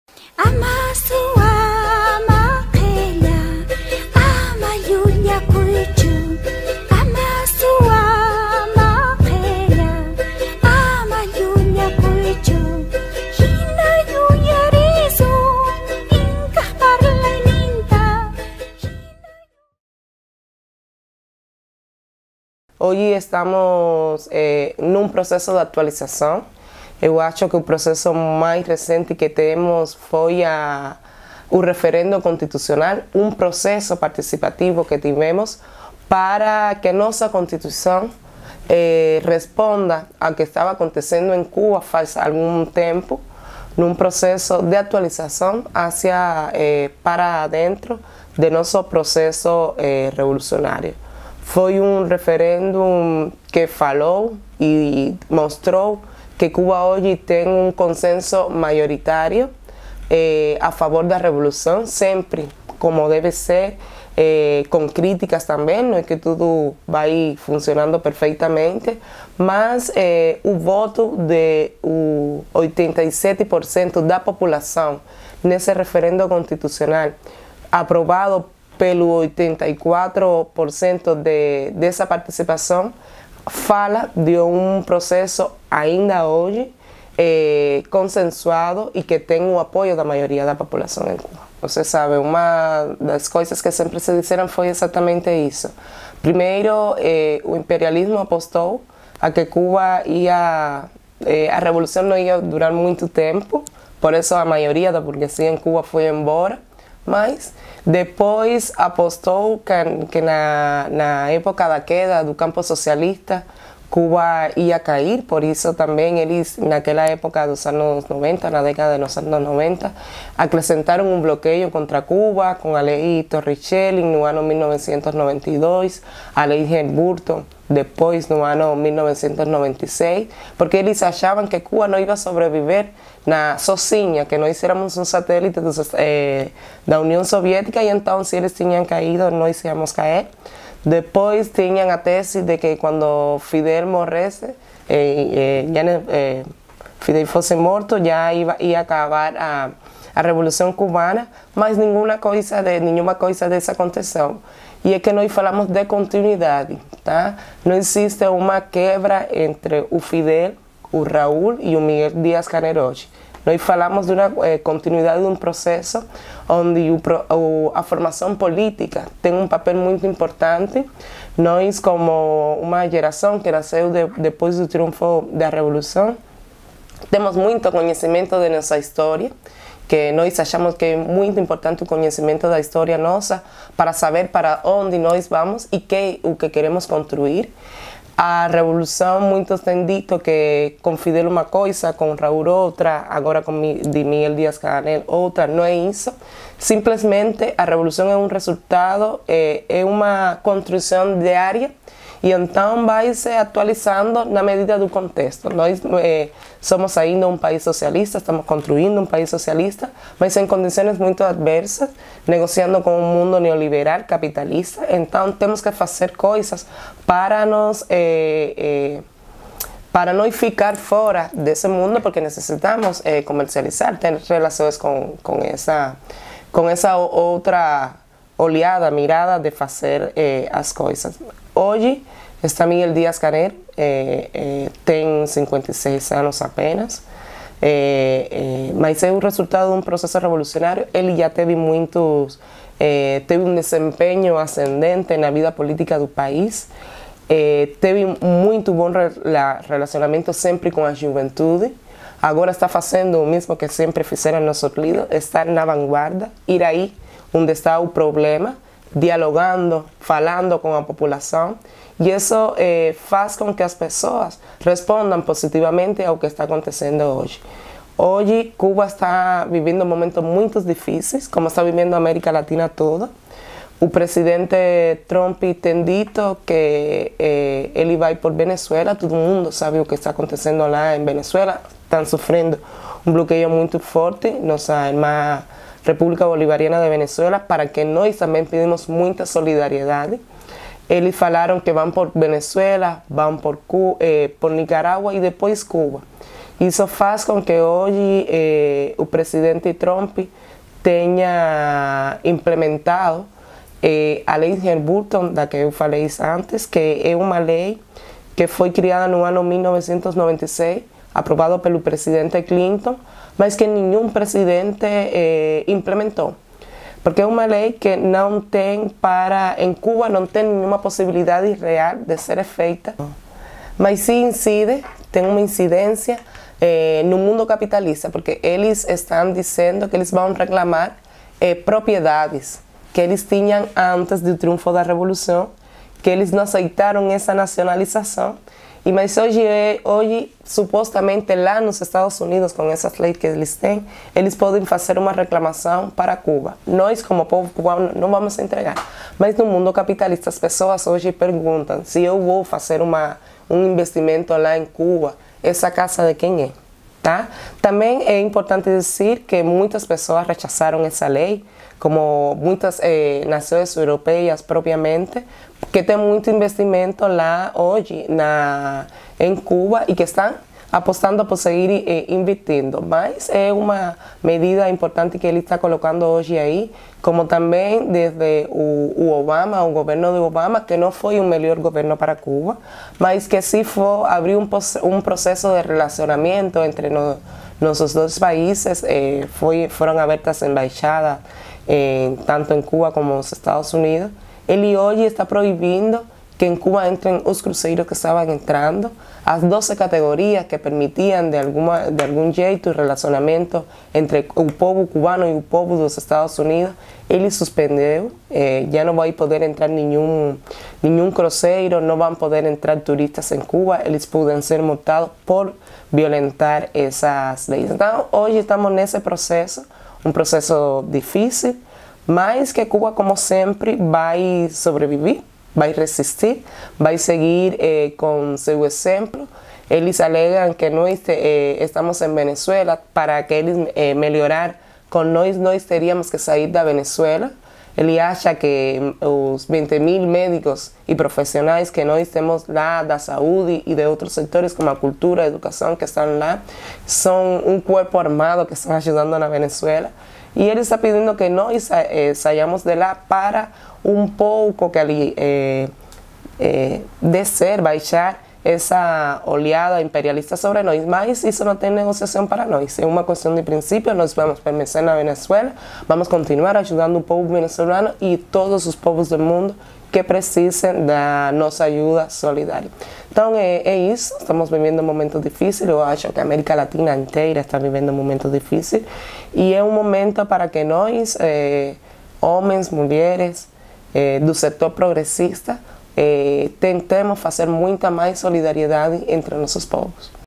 Os novos desafios de Cuba
Entrevista com Yarisleides Medina Vale, do ICAP/Cuba, na qual analisa os desafios de Cuba sob a presidência de Díaz-Canel.